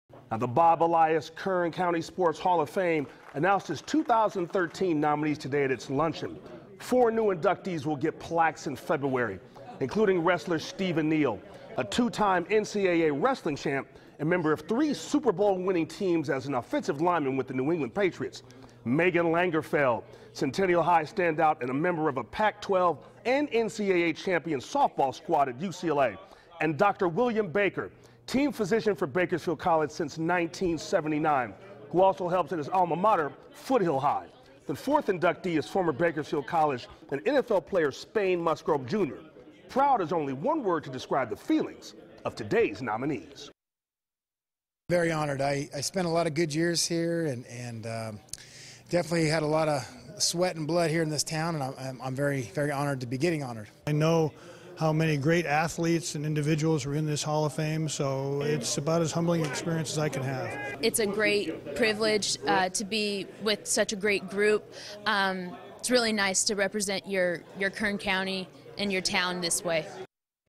Four new nominees to be inducted to Bob Elias Kern County Sports HOF
Stephen Neal, Megan Langenfeld, Dr. William Baker and Spain Musgrove Jr were nominated Thursday for enshrinement into the Bob Elias Kern County Sports Hall of Fame.
Neal was a two-time national wrestling champion for Cal State Bakersfield in 1998 and '99, as well as playing offensive line as a part of three Super Bowl-winning teams for the New England Patriots.
Langenfeld is a Centennial High graduate who helped the softball team to a Valley title in 2004 before going to UCLA and winning a Pac-12 crown in 2